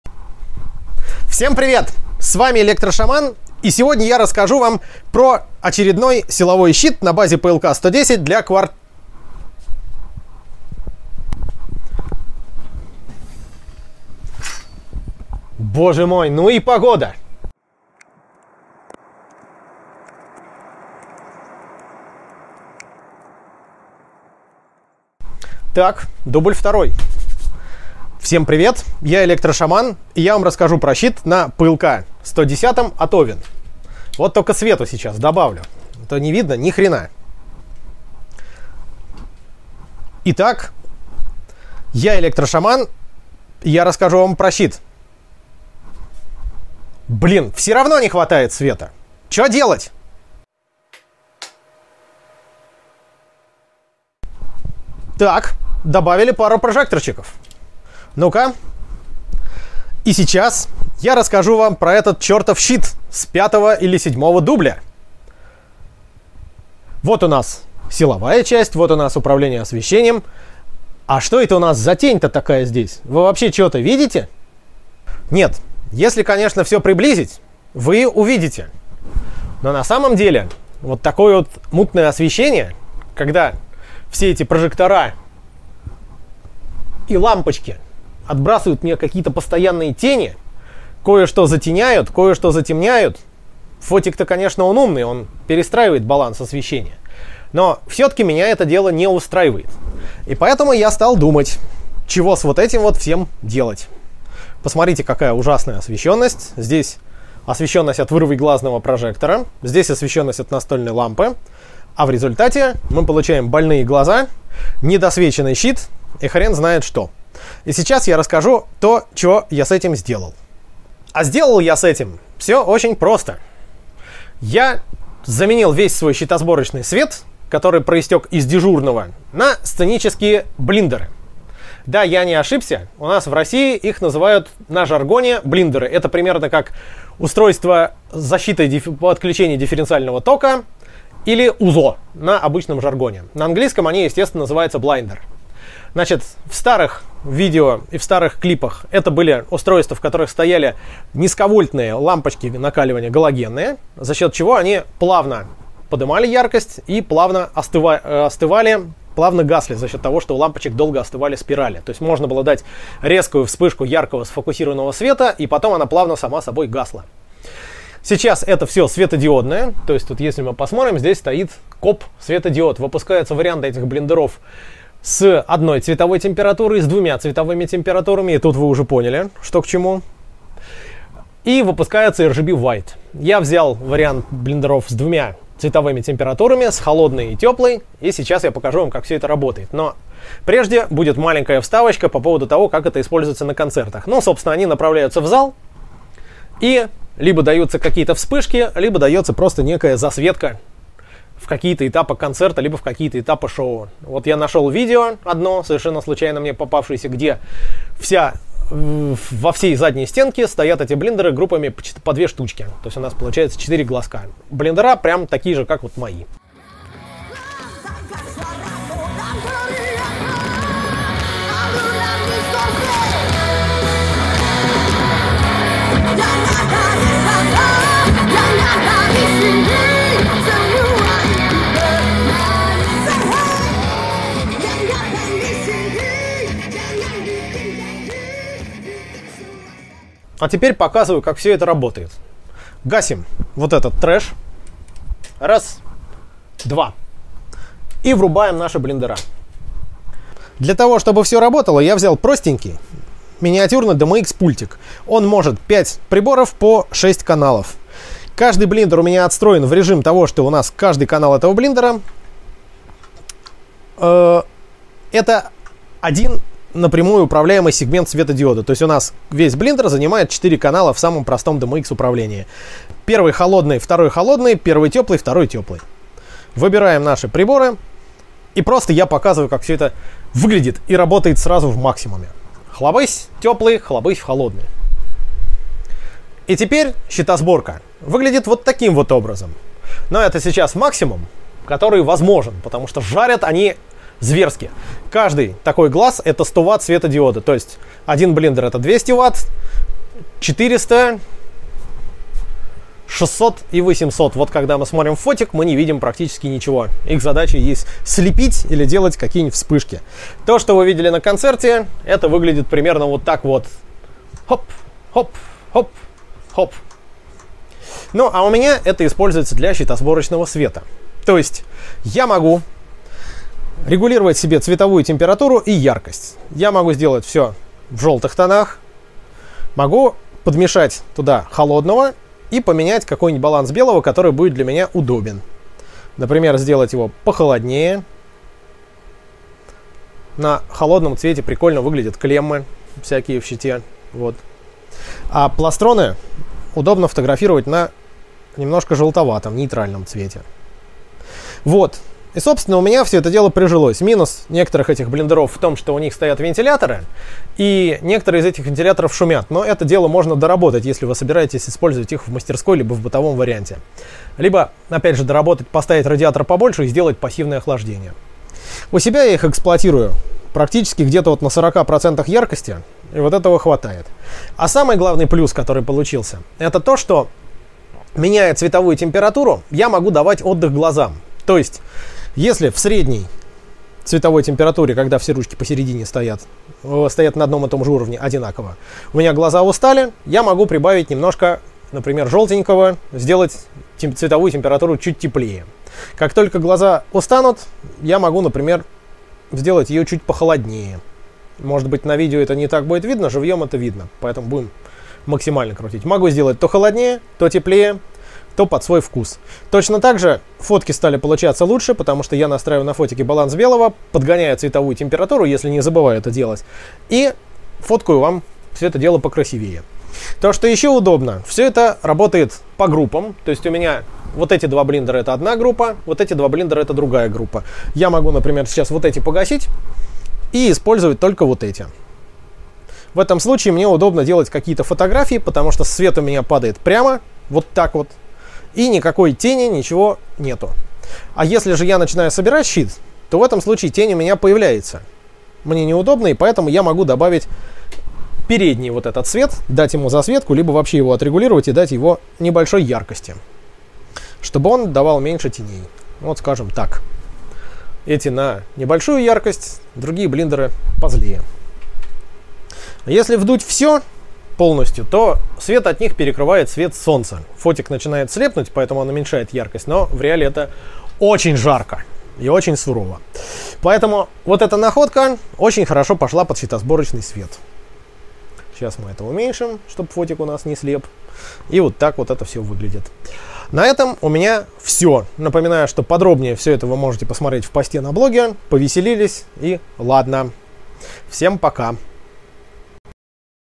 Cs-Cs: Сценические Блиндеры (LED COB Blinder) в качестве рабочего света
Прикупил (в прошлом году) сценические блиндеры COB 200W с двумя цветовыми оттенками: тёплым и холодным. Вообще они используются для заливки сцены, но я сделал из них удобный рабочий свет для сборки щитов или работ на верстаке.
Использована вставка из этого видео Все копирайты этой вставки принадлежат владельцам того видео.